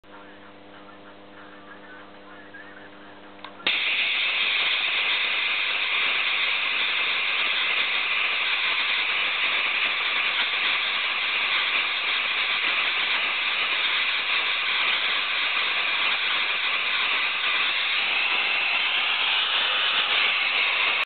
Very nice micro sgtc
Mini new sgtc with MOT and zvs ;)  Flyback give ~30KV, cap have 5-8nF home made, primary coil 4 turn it's all